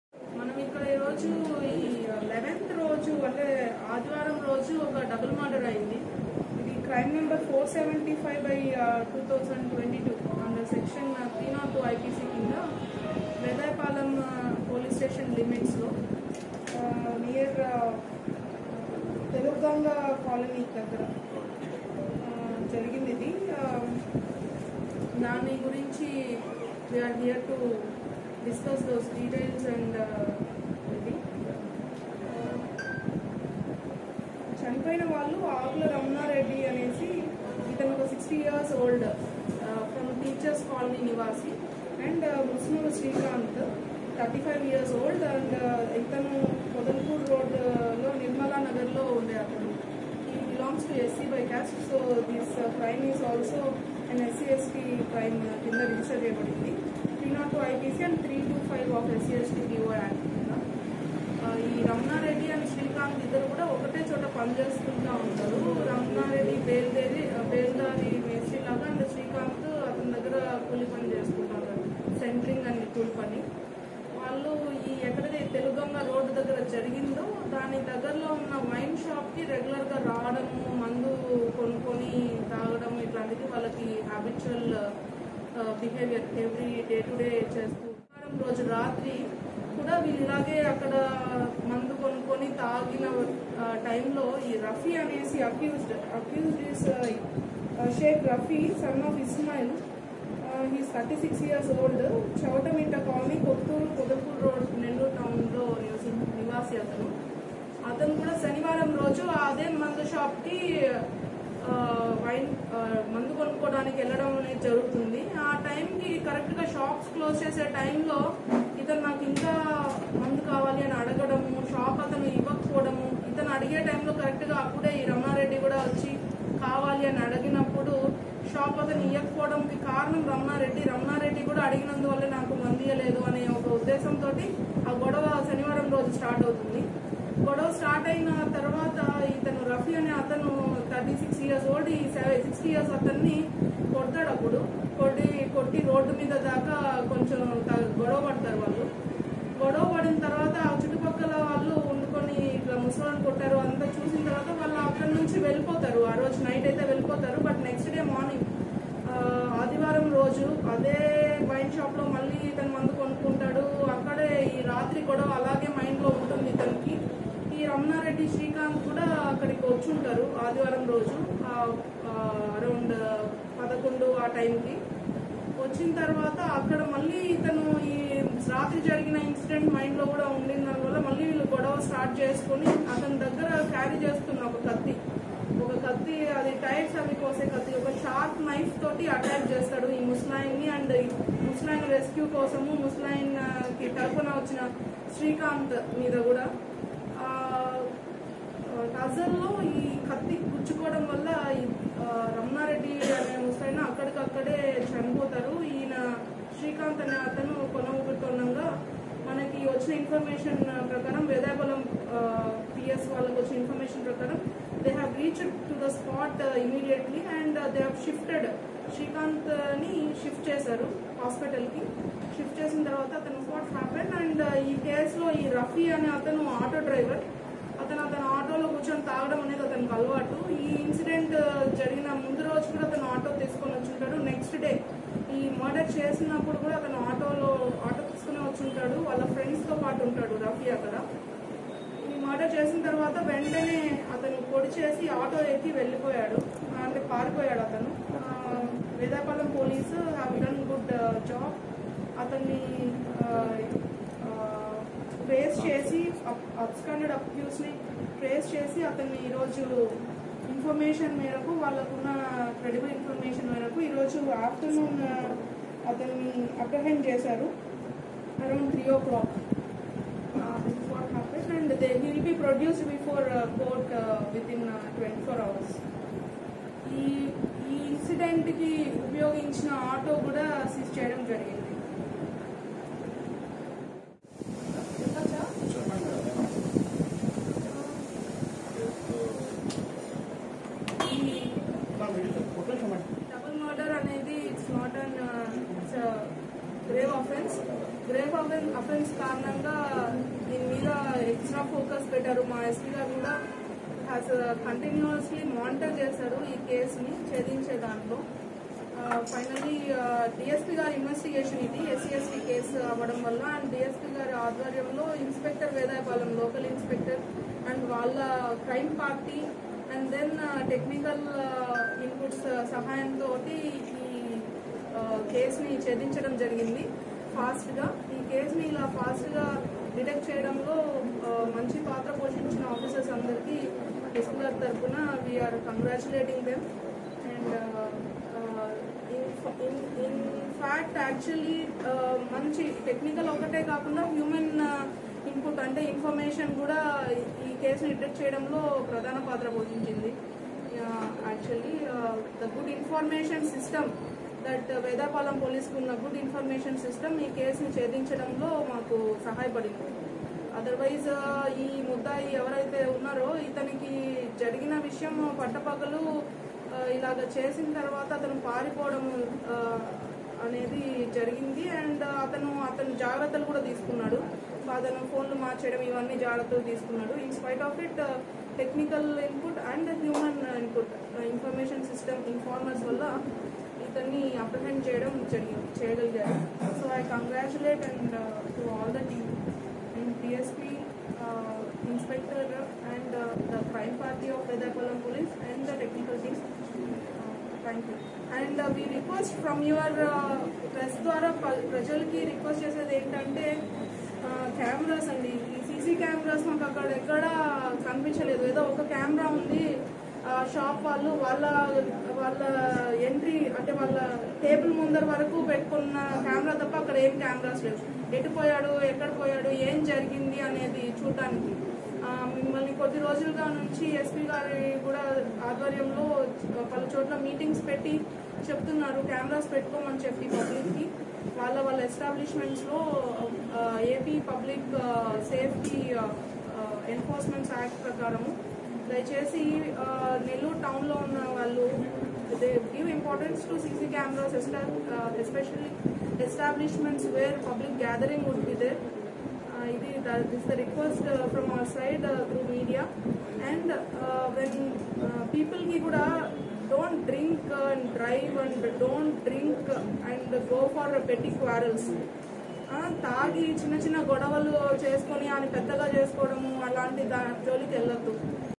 జంట హత్యల కేసులో | ముద్దాయిని అరెస్టు చేసిన | నెల్లూరు పోలీసులు
*SPS నెల్లూరు జిల్లా*

 తాగిన మత్తులో ఎదురించాడని హత్య చేసిన ముద్దాయి..
 శనివారం రాత్రి మద్యం కొనుగోలు సమయంలో ముద్దాయికి, రమణారెడ్డికి వాదులాట.
 గత రాత్రి జరిగిన వాదులాట కారణంగా మరుసటి రోజు ముద్దాయిని ప్రశ్నించిన మృతులు..
 సంఘటన జరిగిన వెంటనే బృందాలను ఏర్పాటు చేసిన జిల్లా యస్.పి. గారు..
 ముగ్గురూ ఒకే ప్రాంతానికి చెందిన వారు.. నిత్యం కలిసి మద్యం సేవించే వారు.
 సాంకేతికత ఆధారంగా ముద్దాయిని చాకచక్యంగా అరెస్ట్ చేసిన వేదాయపాలెం పోలీసులు. nellorepolice @andhrapradeshpolice @dgpandhrapradesh